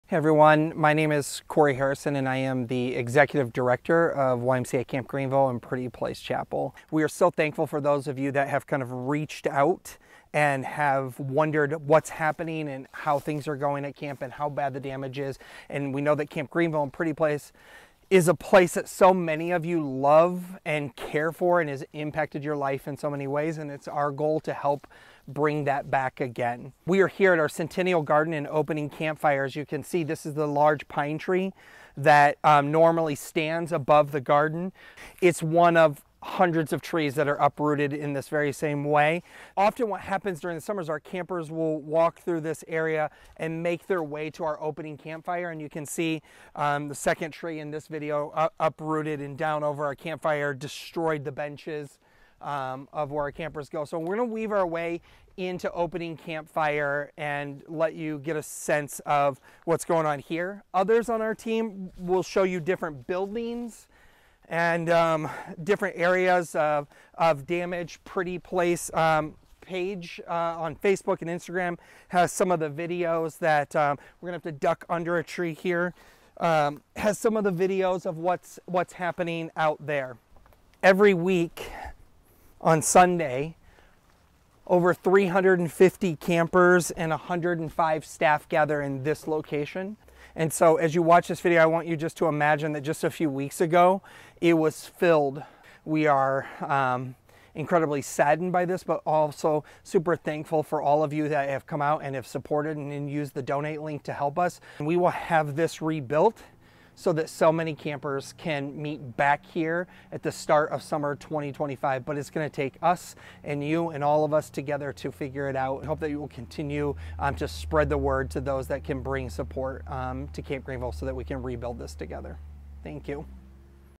Damages to Opening Campfire after Hurricane Helene
This video was recorded Sunday afternoon in hopes to help share how hurricane Helene affected specific areas of camp. Today, Cory Harrison, our Excutive Director, is sharing the Centennial Garden as well as our Opening Campfire.

We are committed to preparing Camp Greenville for Summer 2025.

In the coming days, we hope to share more areas around camp that were significantly damaged due to the storm.